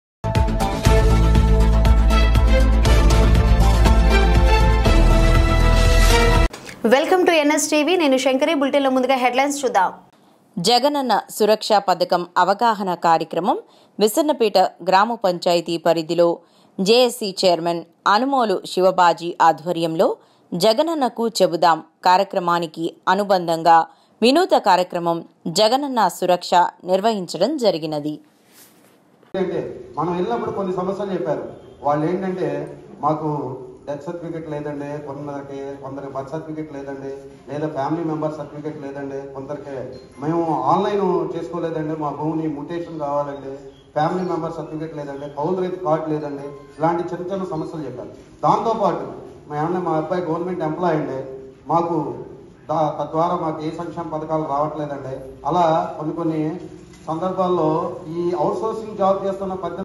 విస్సన్నపేట // జగనన్న సురక్ష పథకం - అవగాహన కార్యక్రమం
జగనన్న సురక్ష పథకం అవగాహన కార్యక్రమం
విస్సన్నపేట గ్రామ పంచాయతీ పరిధిలో  జెఎస్సి చైర్మన్ అనుమోలు శివబాజీ  ఆధ్వర్యంలో జగనన్నకు చెబుదాం కార్యక్రమానికి అనుబంధంగా వినూత కార్యక్రమం "జగనన్న సురక్ష" నిర్వహించడం జరిగింది..